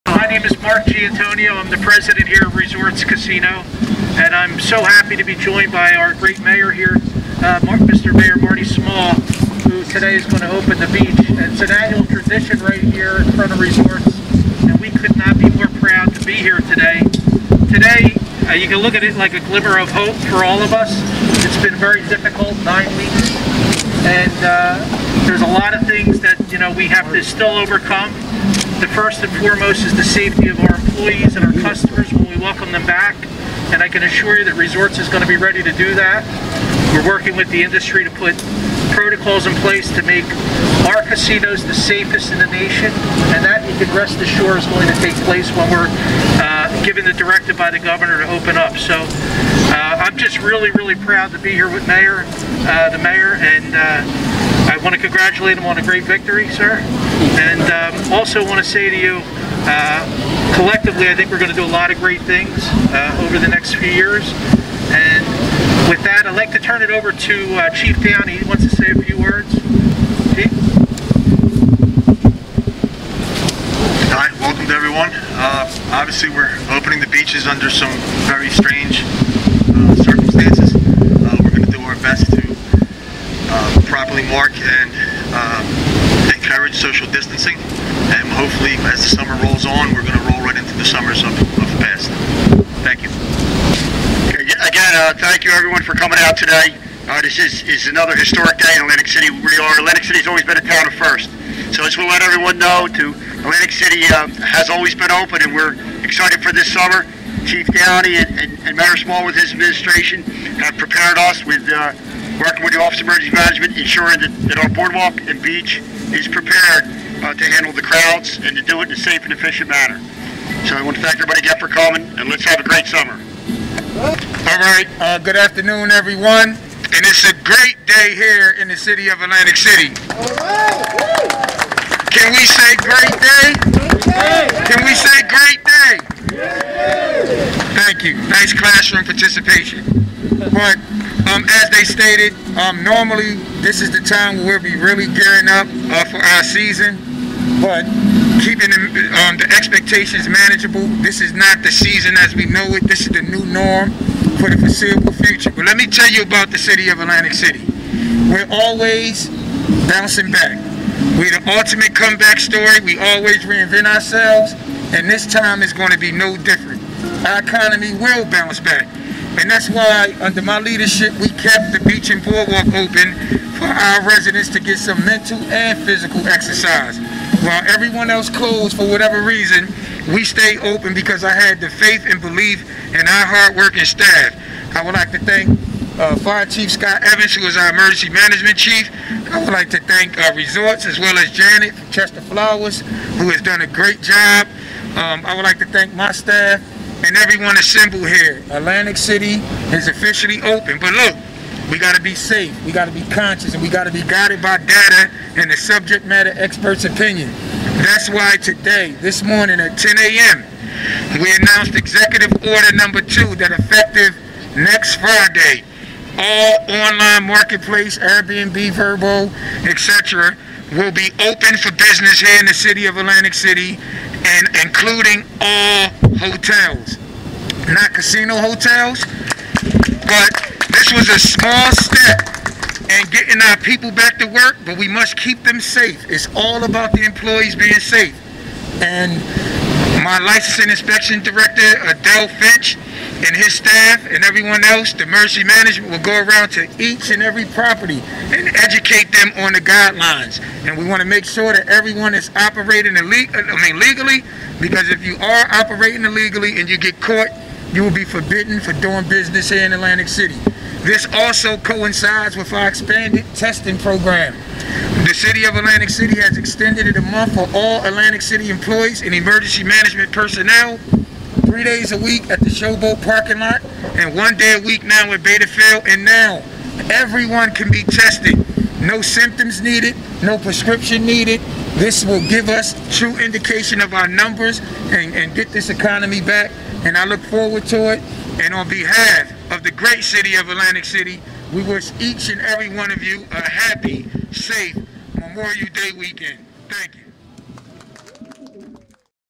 Atlantic City Mayor Marty Small Sr Unlocks the Ocean in Traditional Ceremony
It's a tradition going back years to unlock the ocean in Atlantic City Memorial Day Weekend to official start the Summer season. Today Mayor Marty Small, Resorts President Mark Gianantonio and fire chef Scott Evans gave a few speaches then Mayor Small took to the ocean